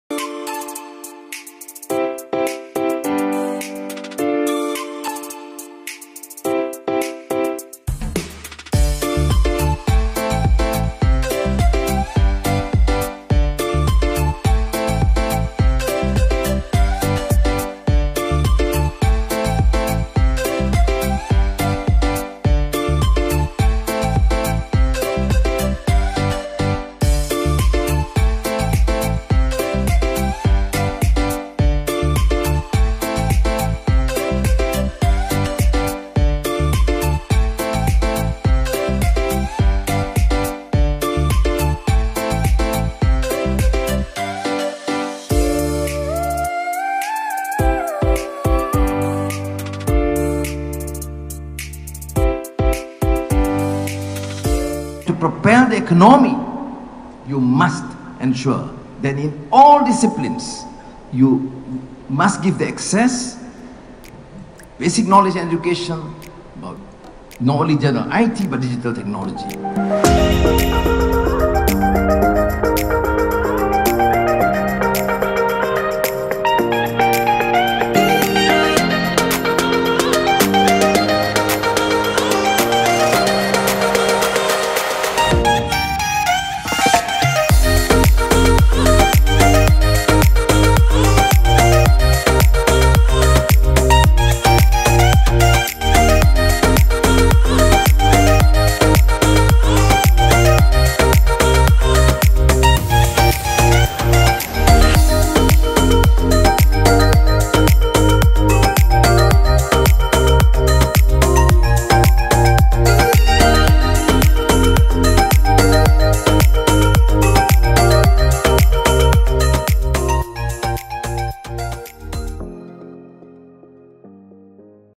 Malaysia Social Media Week (MSMW) 2019
Malaysia Social  Media Week (MSMW) 2019 held at MITEC on 24th  -25th April 2019.